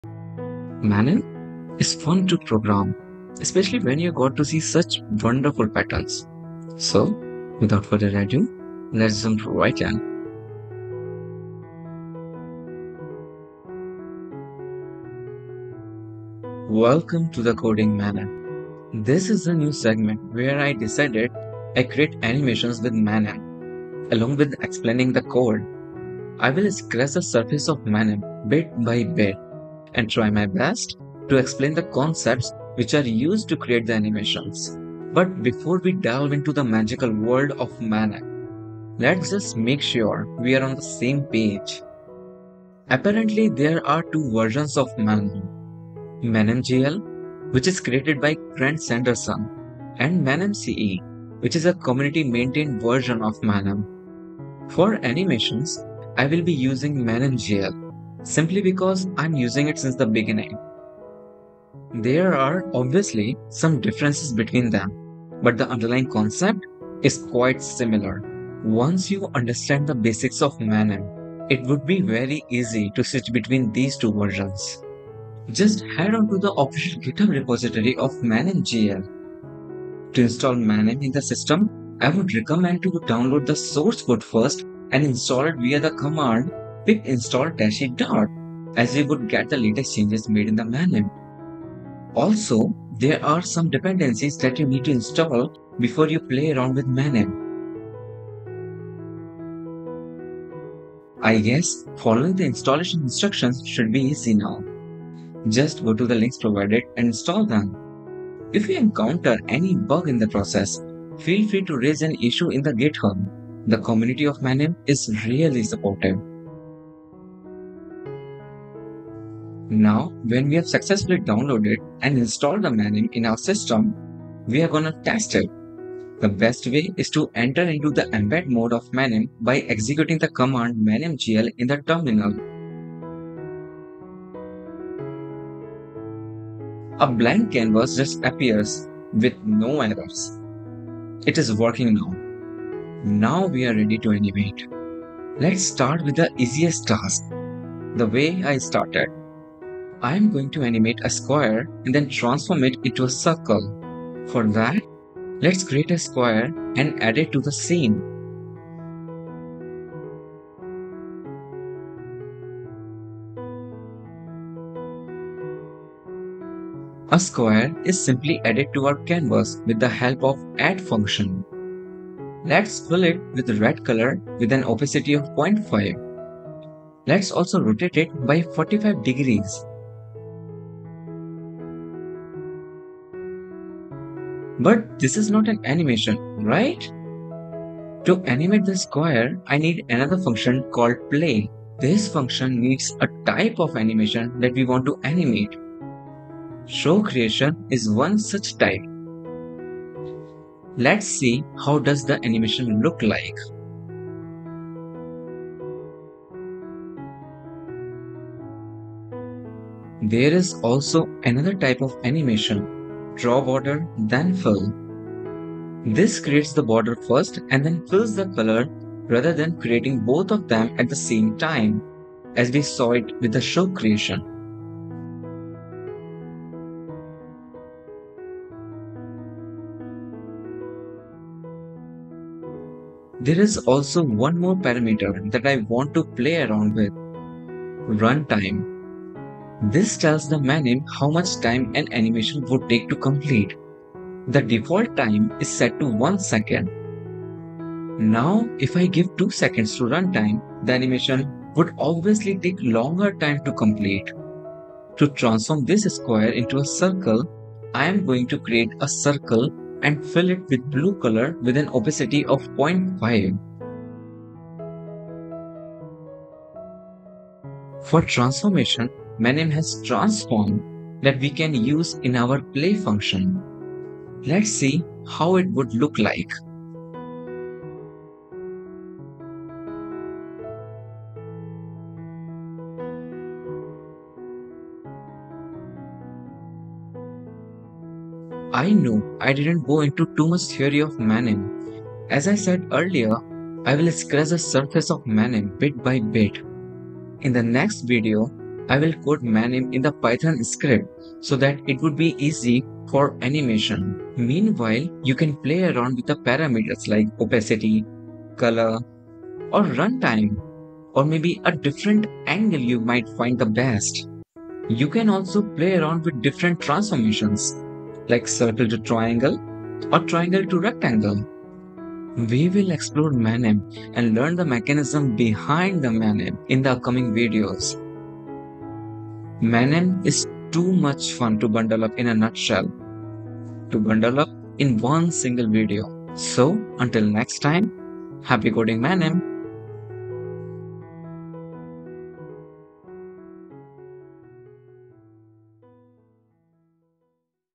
Master Manim 0: Installing & Testing Manim | Manim Explained
CodingManim is a new segment where I would code along with making the animations. I'll try my best to explain the key concepts relevant for animation. In this case, the learning Manim would be so much fun and interactive. If you liked the idea, Welcome Aboard!

Chapters:
0:00 I asked Reddit a Question
0:15 Why Manim?
0:26 Logo Intro
0:31 Welcome to the CodingManim
0:56 Two version of Manim
1:30 Installing ManimGL
2:14 Testing Manim by Making an Animation
5:02 Outro & Thanks for Watching

This video is animated using Grant Sanderson (@3blue1brown)  library Manim (based on Python).
Manim is an engine for precise programmatic animations, designed for creating explanatory math videos.